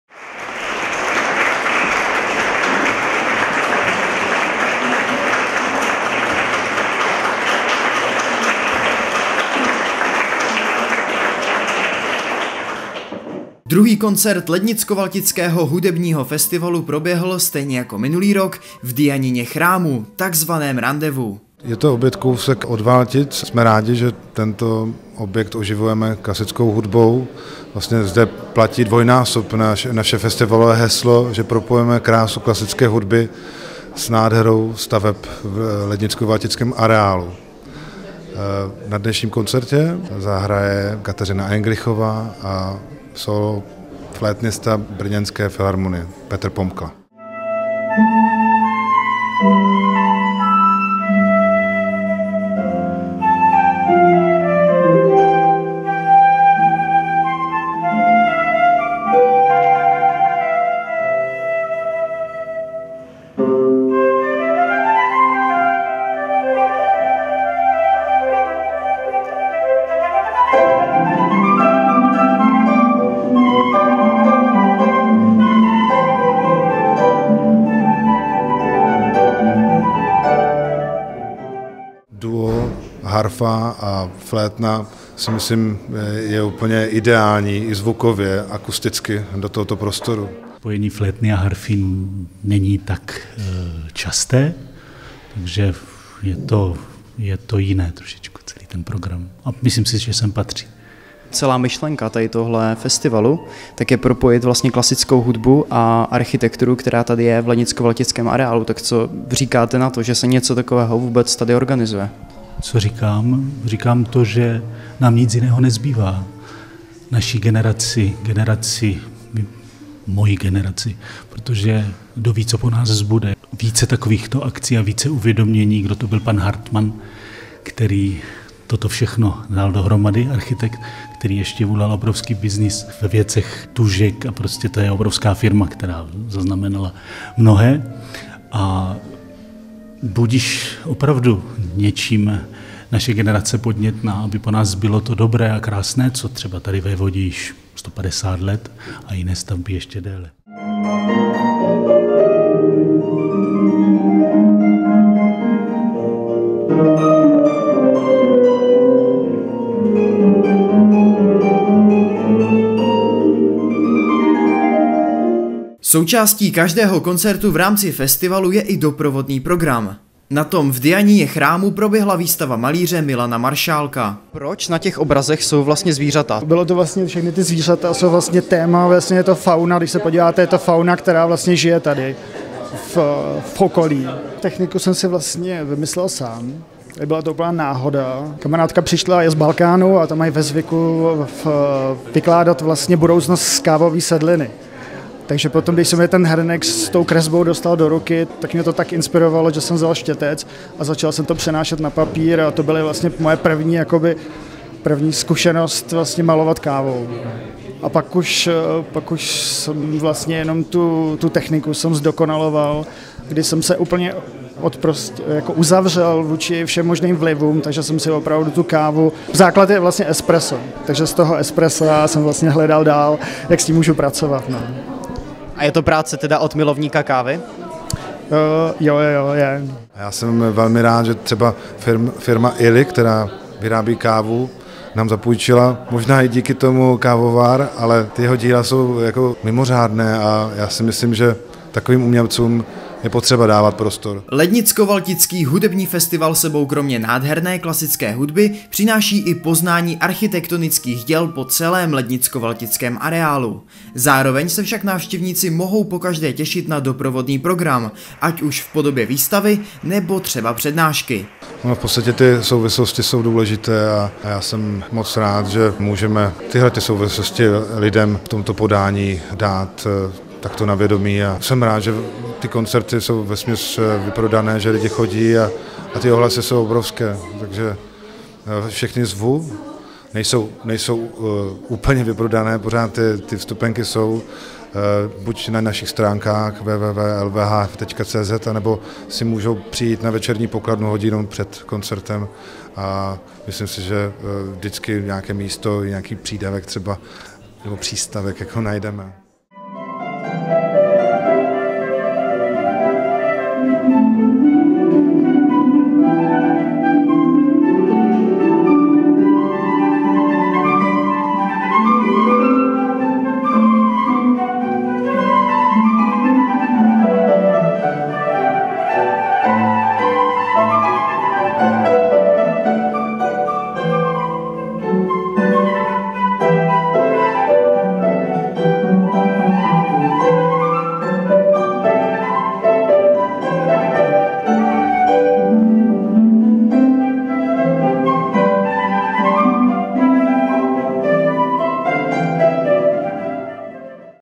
Překrásné spojení klasické hudby a výstavy obrazů v Rendez-Vous v rámci festivalu LVHF
Druhý koncert Lednicko - Valtického hudebního festivalu proběhl, stejně jako minulý rok v Dianině
chrámu, tzv. Rendez-Vous.